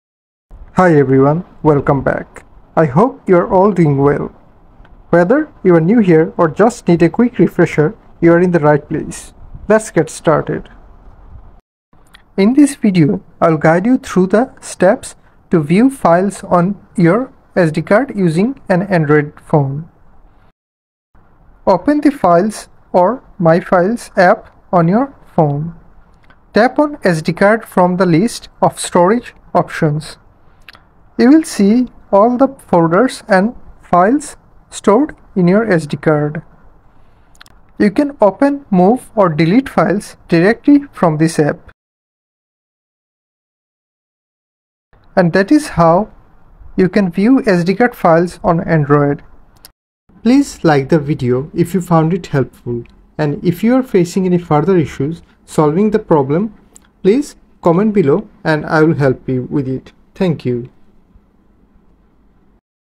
How To View SD Card Files On Android | Full Guide
Hey, this video shows you how to view SD card files on android. Follow the easy steps.